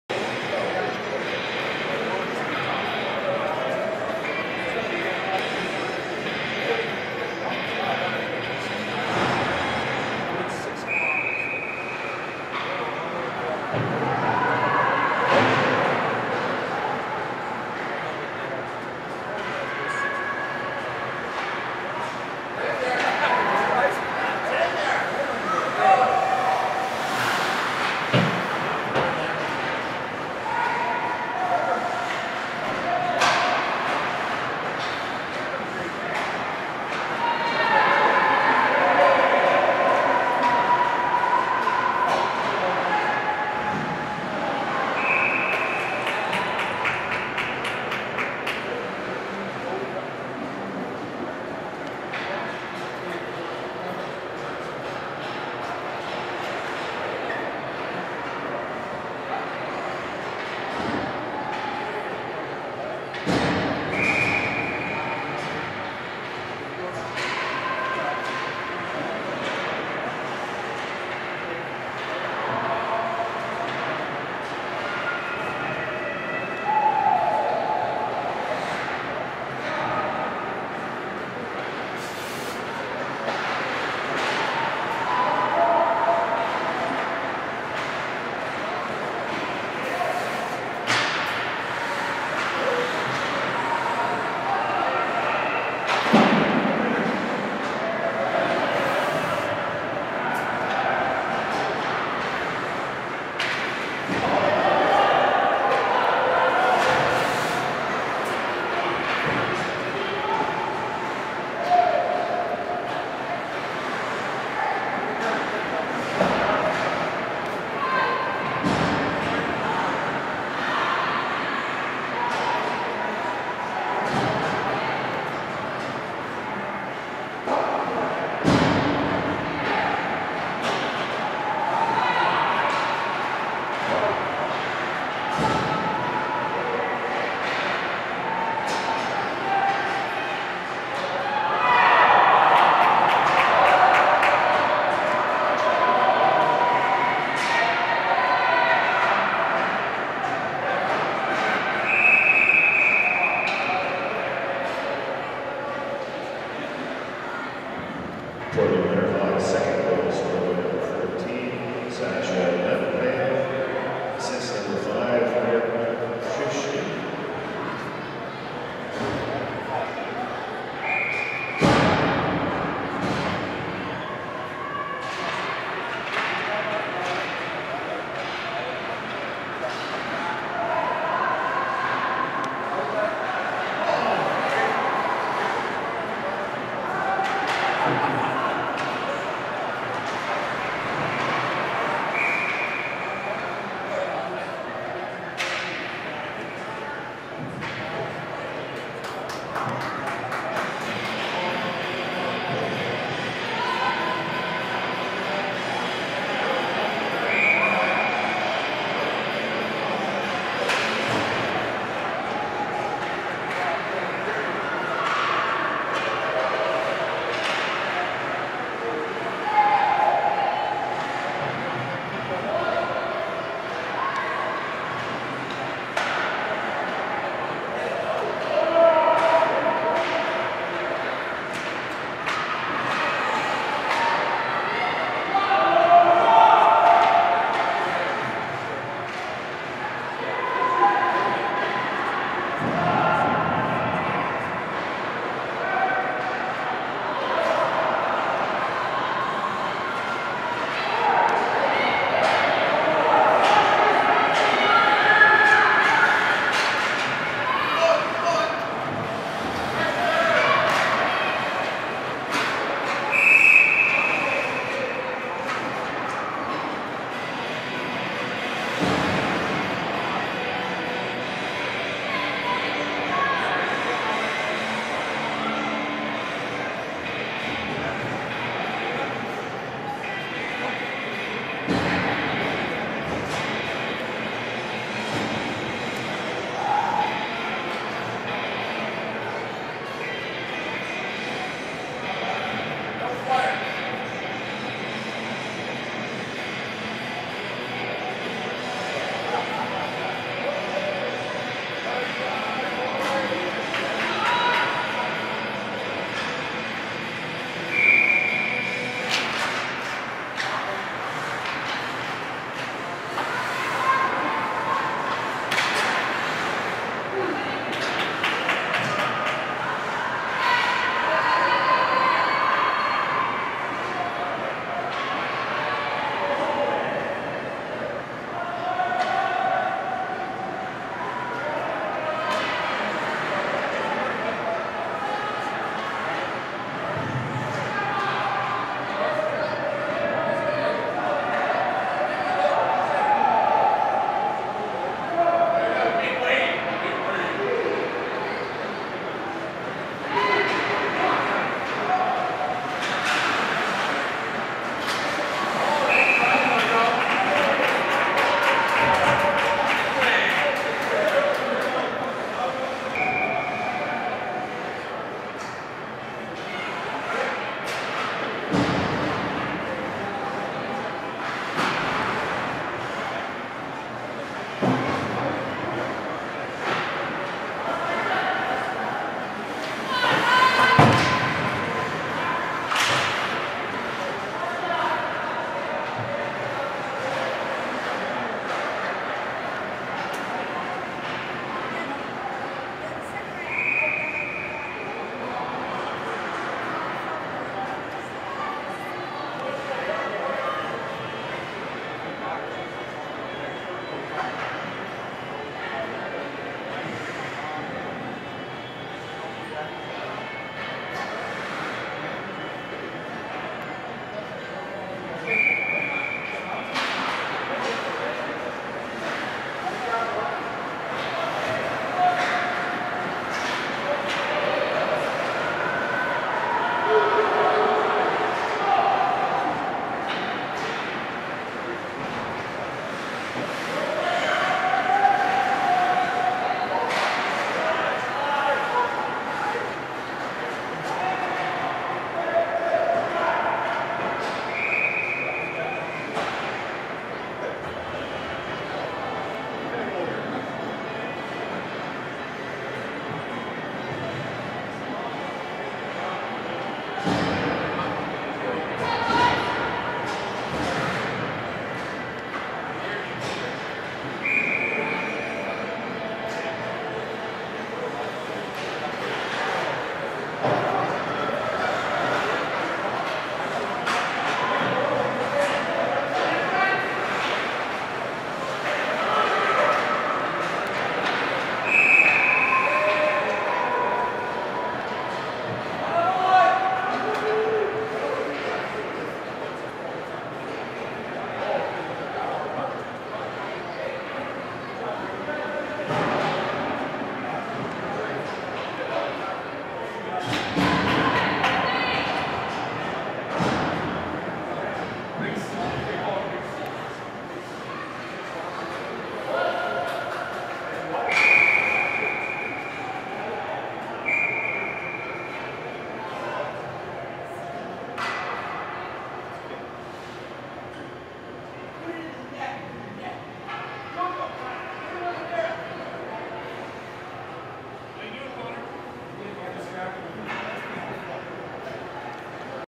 11-10-2012 3rd Period Part 2 vs Juan De Fuca (2012 Port Moody Remembrance Day Tourney)
2012 Port Moody Remembrance Day Tournament. Game between the Portland Jr Hawks Peewee AA team and the Juan de Fuca Jr Grizzlies. Game played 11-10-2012 in Port Moody, BC.